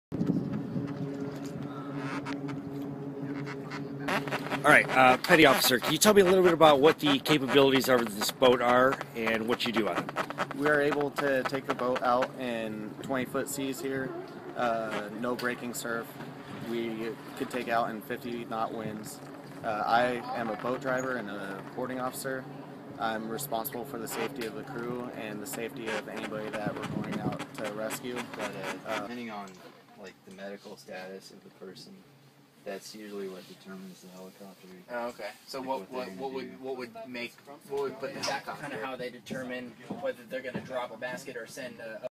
Petty Officer Sean Regan of the @USCG talks about Monterey's 47-foot Motor Lifeboat.
By: Phil Molnar
Published on: August 29, 2014